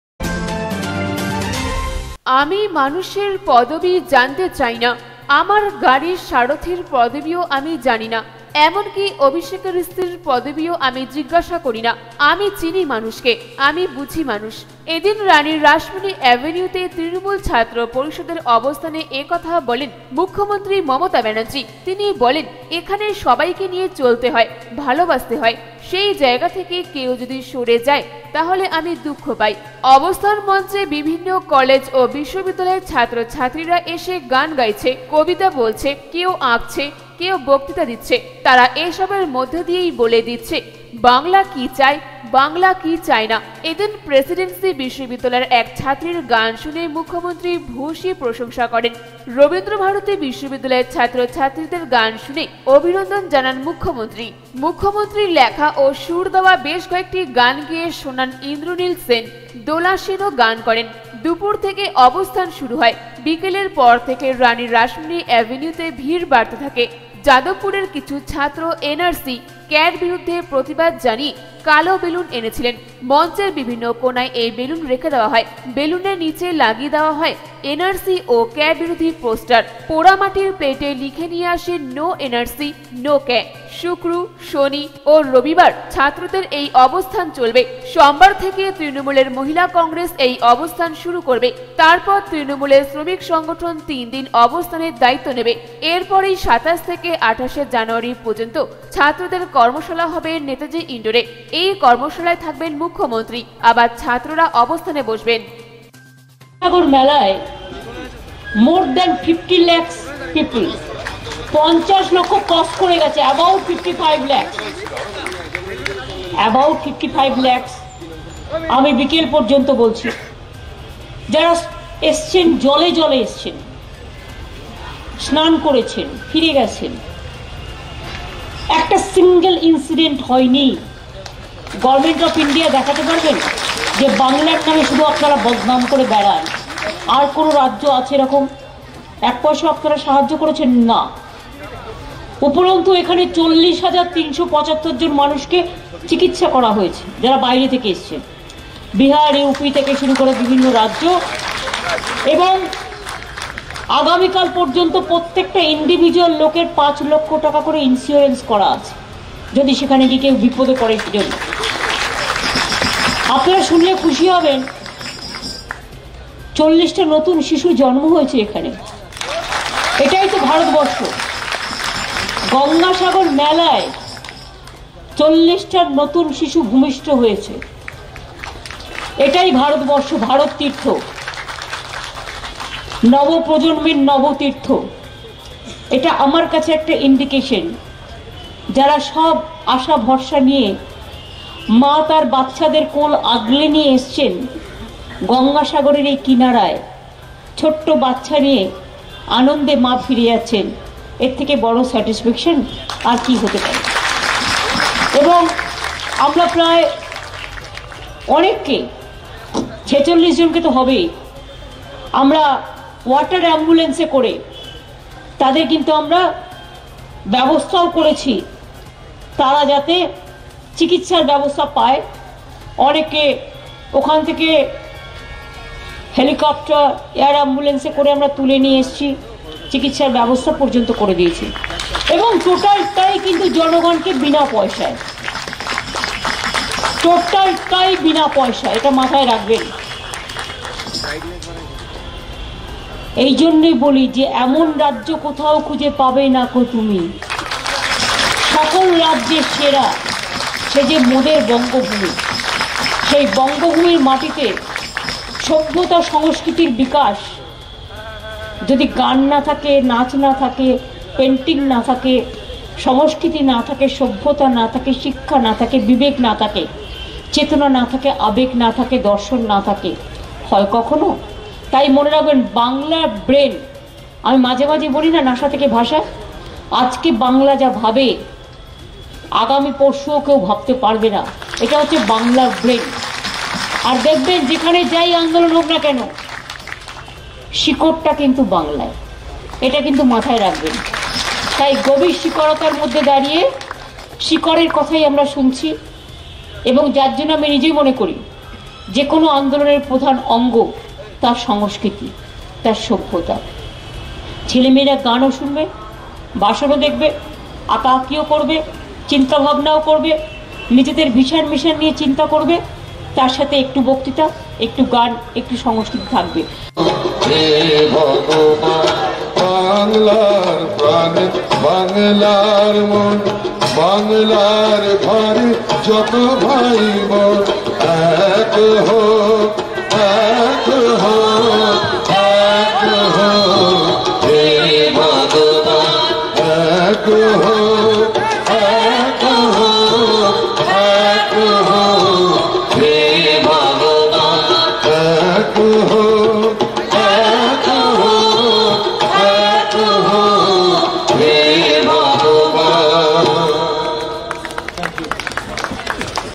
সিএএ এবং এনআরসি র প্রতিবাদে রানী রাসমণি রোডে তৃণমূল ছাত্র পরিষদের ধর্নামঞ্চে মুখ্যমন্ত্রী
সিএএ এবং এনআরসি-র প্রতিবাদে রানী রাসমণি রোডে  তৃণমূল ছাত্র পরিষদের ধর্নামঞ্চে মুখ্যমন্ত্রী মমতা ব্যানার্জি